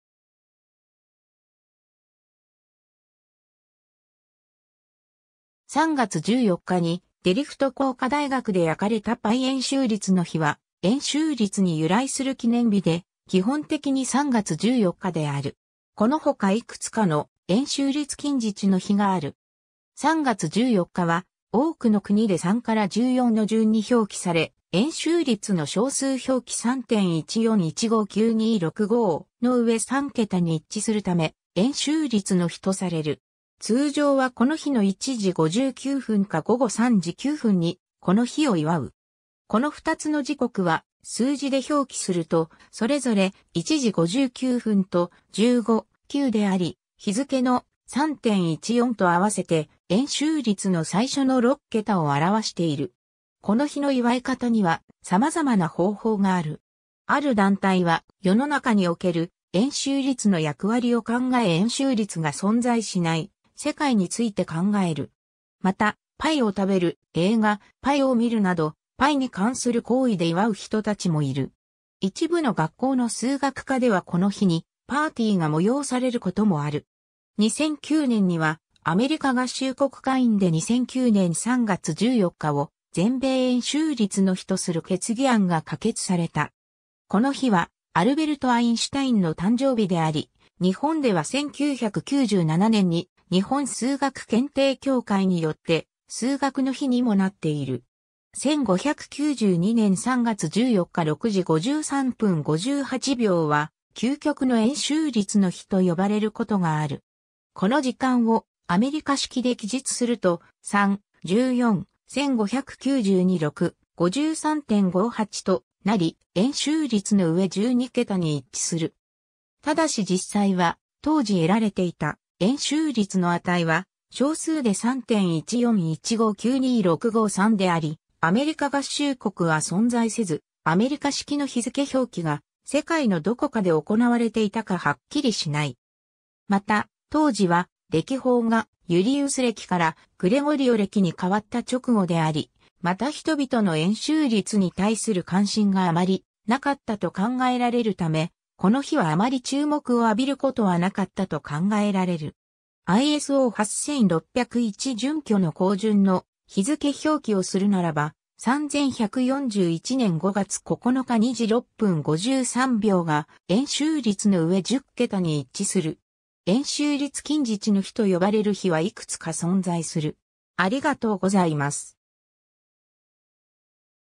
円周率の日
3月14日にデルフト工科大学で焼かれたパイ 円周率の日（えんしゅうりつのひ）は円周率に由来する記念日で、基本的に3月14日である。
このほかいくつかの「円周率近似値の日」がある。
 3月14日は、多くの国で 3-14 の順に表記され、円周率の小数表記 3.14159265... の上3桁に一致するため、「円周率の日」とされる。
 通常はこの日の1時59分か午後3時9分（15時9分）にこの日を祝う。
この2つの時刻は数字で表記するとそれぞれ「1:59」と「15:9」であり、日付の「3.14」と併せて円周率の最初の6桁を表している。
(π=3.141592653…) この日の祝い方には様々な方法がある。
 ある団体は、世の中における円周率の役割を考え円周率が存在しない世界について考える。
また、パイを食べる・映画「π」を見るなど「パイ」に関する行為で祝う人たちもいる。
一部の学校の数学科ではこの日にパーティーが催されることもある。
 2009年にはアメリカ合衆国下院で2009年3月14日を「全米円周率の日」とする決議案が可決された。
 この日はアルベルト・アインシュタインの誕生日であり、日本では1997年に日本数学検定協会によって「数学の日」にもなっている。
 1592年3月14日6時53分58秒は、究極の円周率の日（時間）と呼ばれることがある。
この時間をアメリカ式で記述すると、3/14/1592 6:53.58 となり、円周率の上12桁に一致する。
 ただし実際は、当時得られていた円周率の値は小数で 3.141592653 であり、アメリカ合衆国は存在せずアメリカ式の日付表記が世界のどこかで行われていたかはっきりしない。
また、当時は暦法がユリウス暦からグレゴリオ暦に変わった直後であり、また人々の円周率に対する関心が余りなかったと考えられるため、この日はあまり注目を浴びることはなかったと考えられる。
 ISO 8601 準拠の降順の日付表記をするならば、3141年5月9日2時6分53秒が円周率の上10桁に一致する。
 円周率近似値の日（えんしゅうりつきんじちのひ）と呼ばれる日はいくつか存在する。